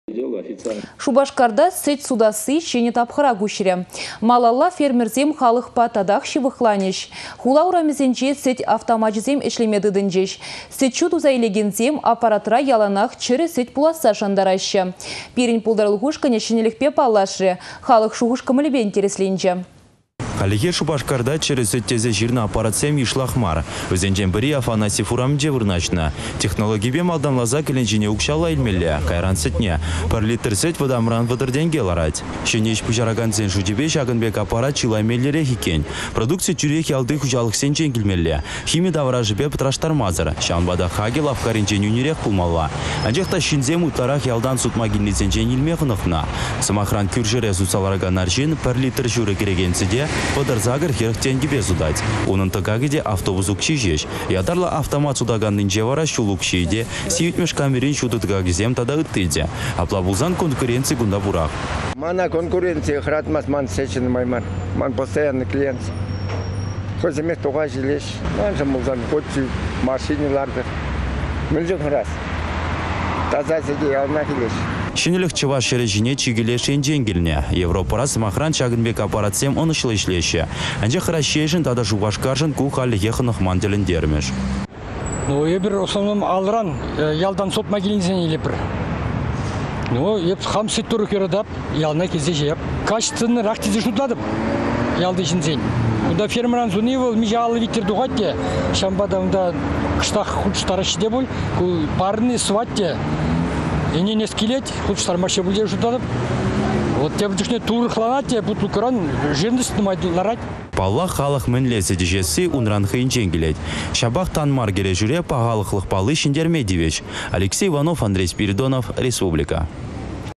Сĕт кашни утăмра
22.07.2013
Шупашкарта сĕт сутасси çĕнĕ тапхăра куçрĕ. Малалла фермерсем халăхпа татах çывăхланаççĕ. Хула урамĕсенче ятарлă «молокоматсем» ĕçлеме тытăнчĕç. Сĕт-çу туса илекенсем аппаратра яланах чĕрĕ сĕт пуласса шантараççĕ.  Пирĕн пултарулăх ушкăнĕ çĕнĕлĕхпе паллашрĕ - халăх шухăш-кăмăлĕпе интересленчĕ.